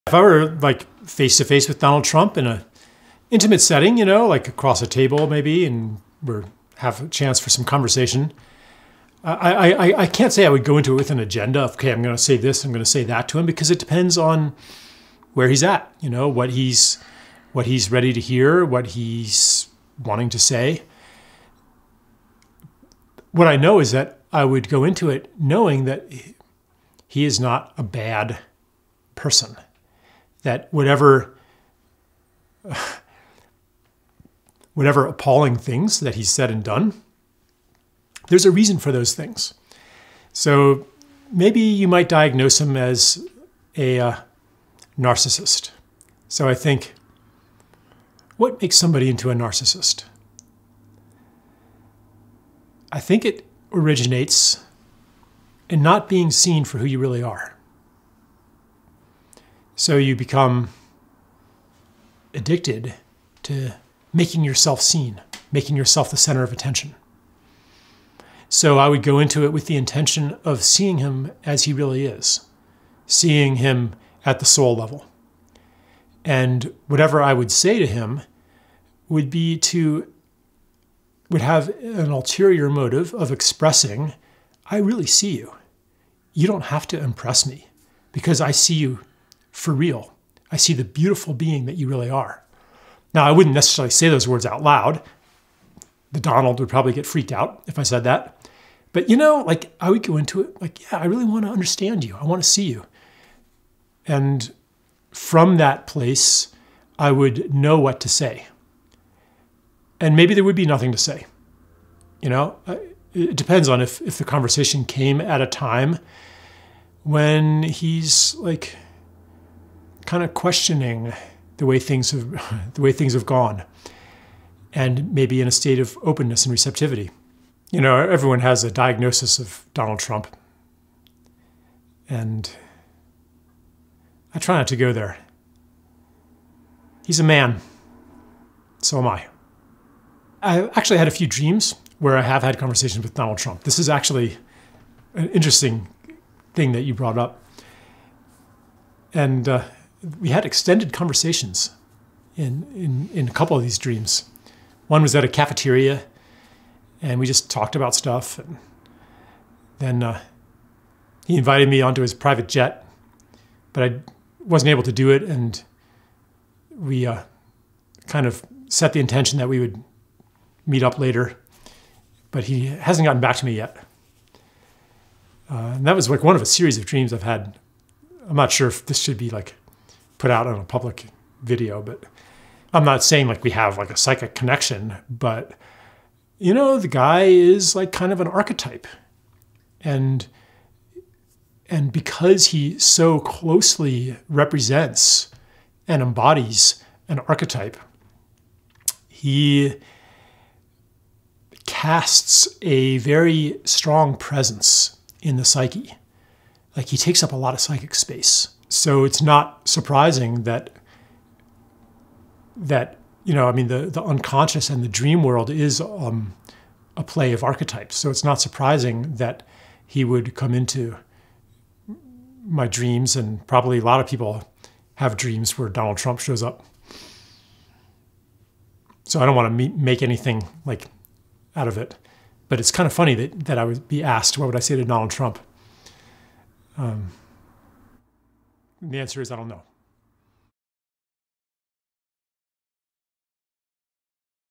My (Theoretical) Meeting with Donald Trump
If I were face-to-face with Donald Trump in an intimate setting, I would go into it knowing that he's not a bad person. Whatever appalling things he's said and done - there's a reason for those things.

From the course Unlearning: For Change Agents, offered in the spirit of the gift at CharlesEisenstein.org/unlearning